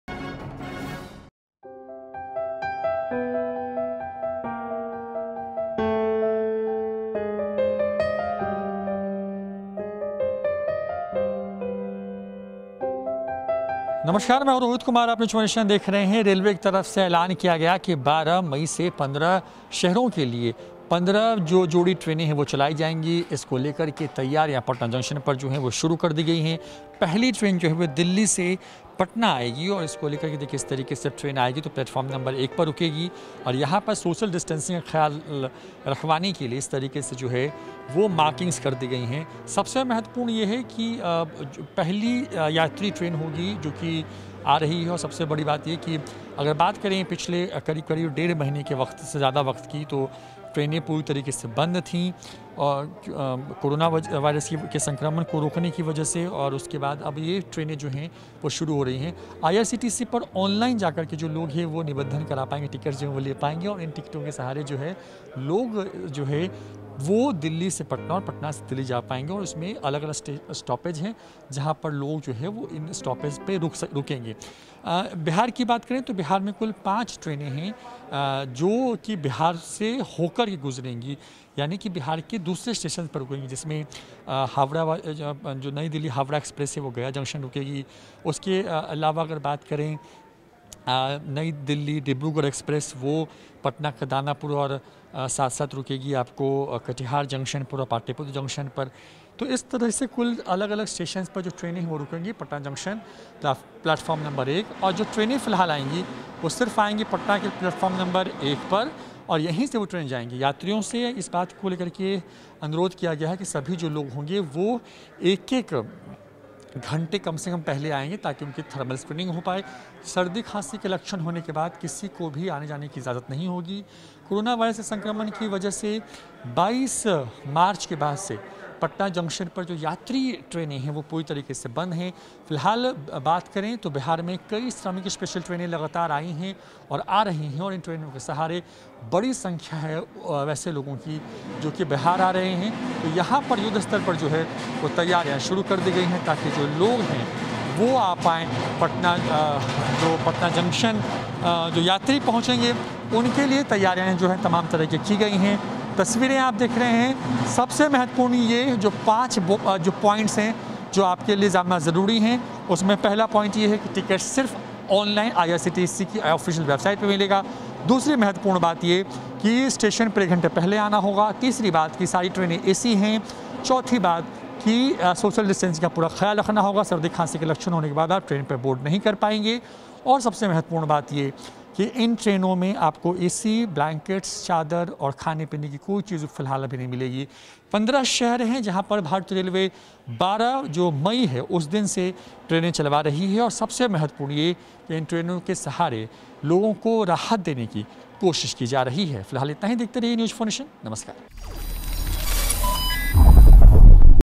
Patna से Delhi के लिए 12 May से चलने वाली Rajdhani Express के लिए Patna Junction पर तैयारी पूरी
Patna से Delhi के लिए 12 May से चलने वाली Rajdhani Express के लिए Patna Junction पर तैयारी पूरी  | News4Nation